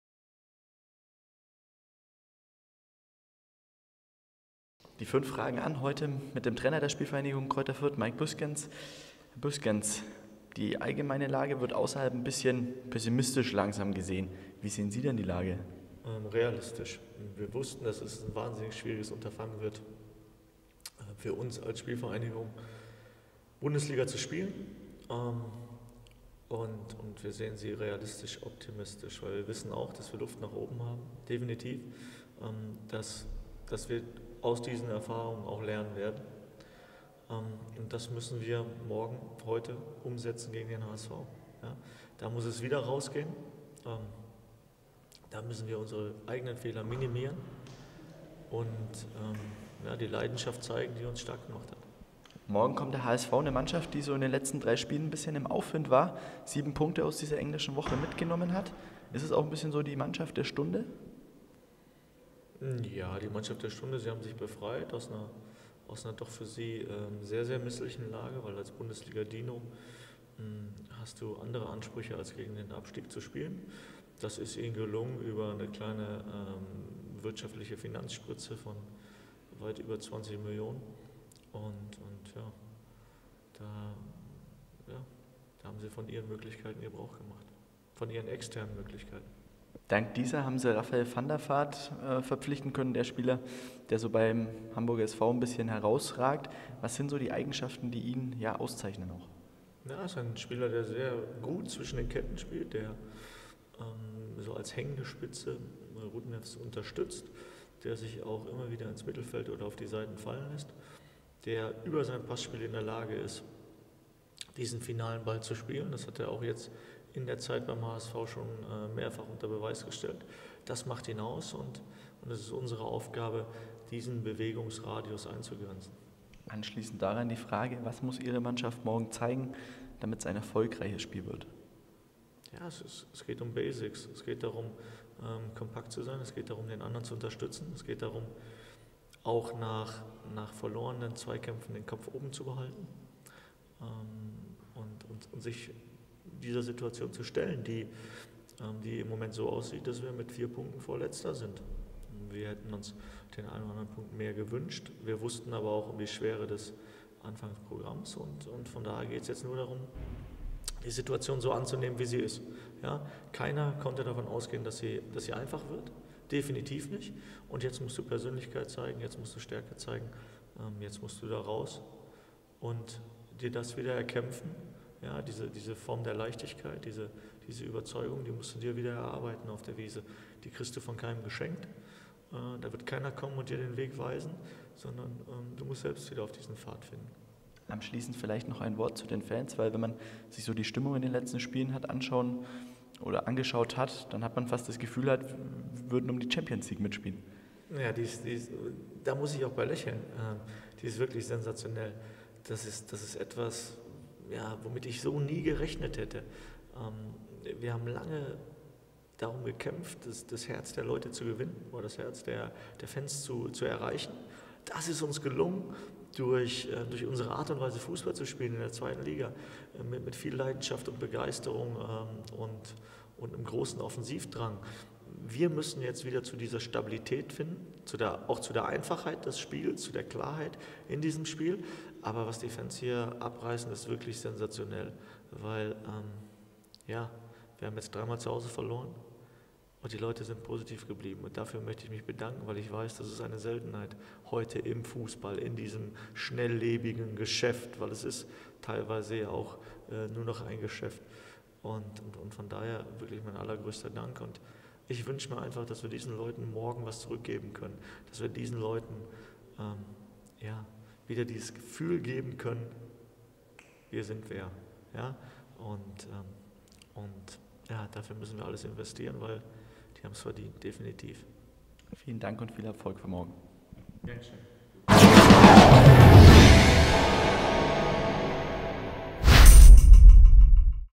5 Fragen an Mike Büskens
Heute haben wir unsere 5 Fragen an den Cheftrainer der SpVgg Greuther Fürth gestellt. Dieser blickt auf das morgige Spiel gegen den HSV und bedankt sich für die großartige Unterstützung während der ersten drei Heimspiele. Viel Spaß.